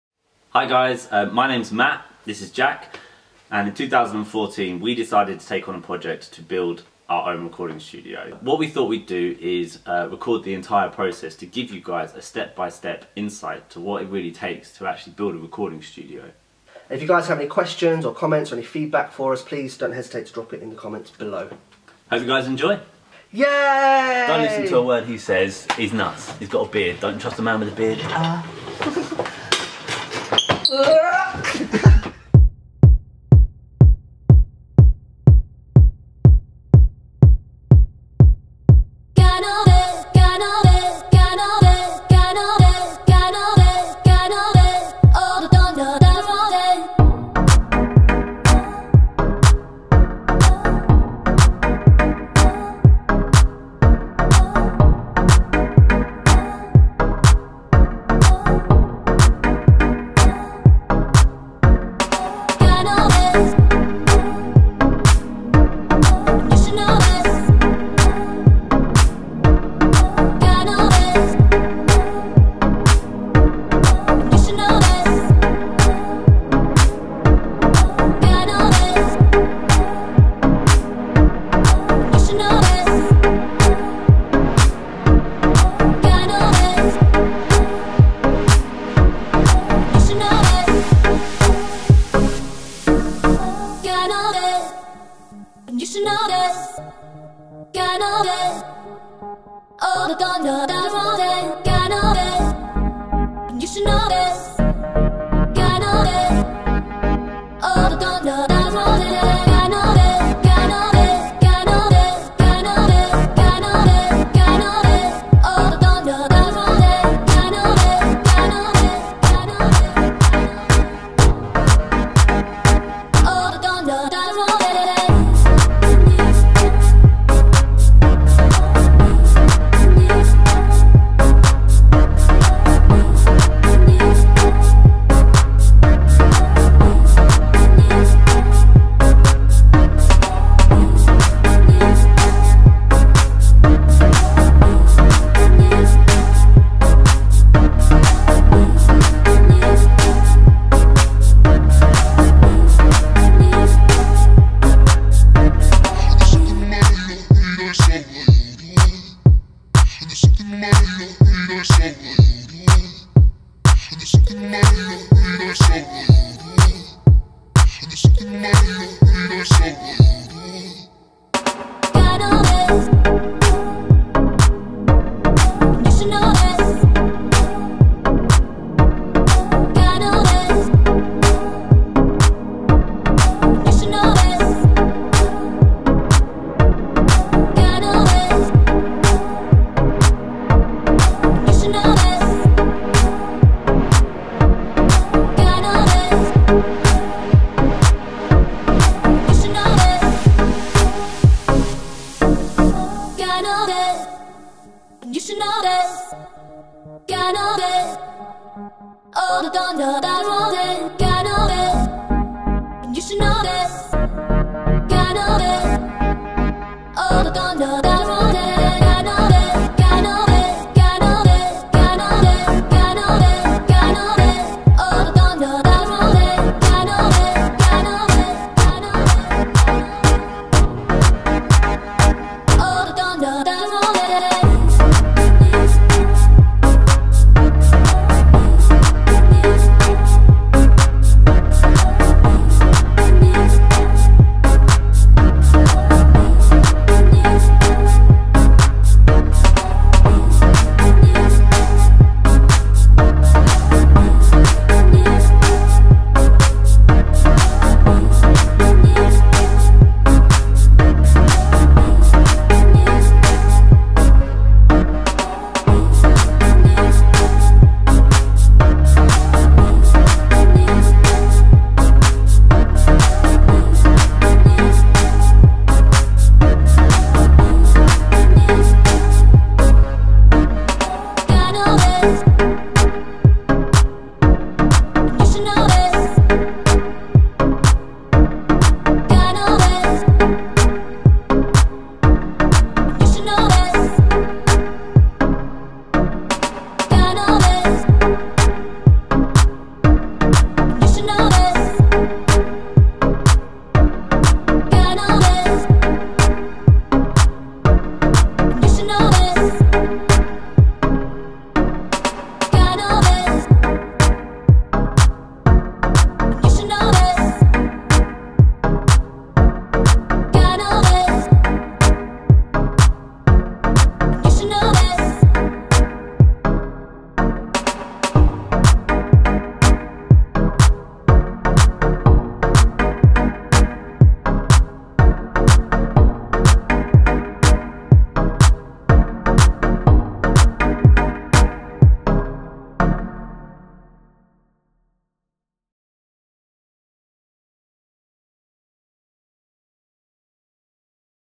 Soundbox Studios - How to build a recording studio
Soundbox Studios was built from the ground up and heres a video to show you just how we did it!

The project took four months from start to finish.

Special thanks to:

Mike Thompson
Kevin Smith
Shane Nyman
Rebecca James
Ayinde 'Index' Gardiner
Will Anderson
Scott Matthews
Ryan Samuda
Steve Wood
Laura Wood
William Sachiti
George Nyman
Dan Williams
Sean James
Rob James
Geraldine James
Rhys James
Elizabeth Cooling
Richard Cooling
Dave Prytherc
Suzanne Prytherc